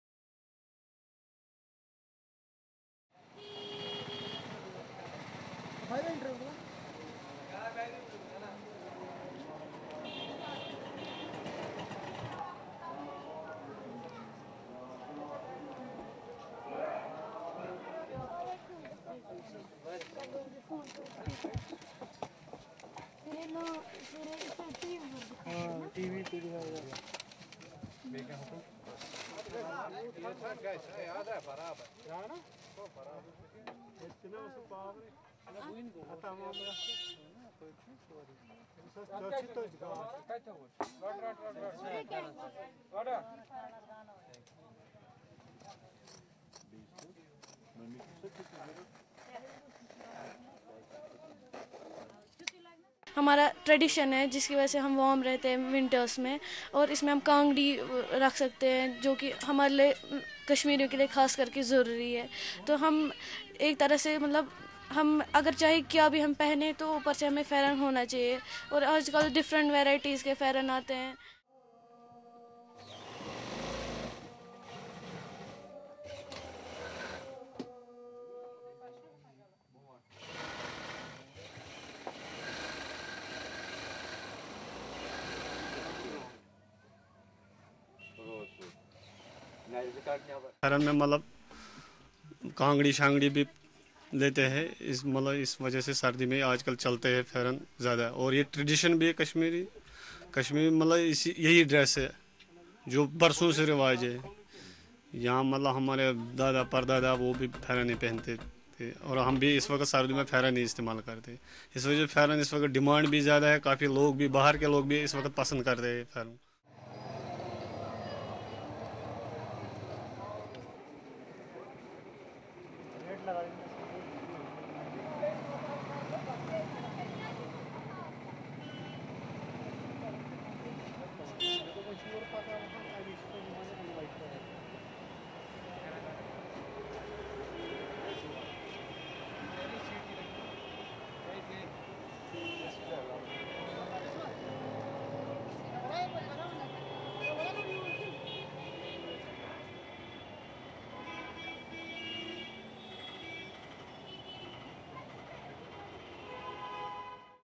13 Jan, 2014 - Traditional robe sales buzzing clothing markets
13 Jan, 2014 (ANI) - Clothing markets across India's northern sate of Jammu and Kashmir are buzzing with the surge in sales of traditional knee-length robe "Pheran" treasured most during bone chilling winters, as it brings warmth shielding people from intense cold.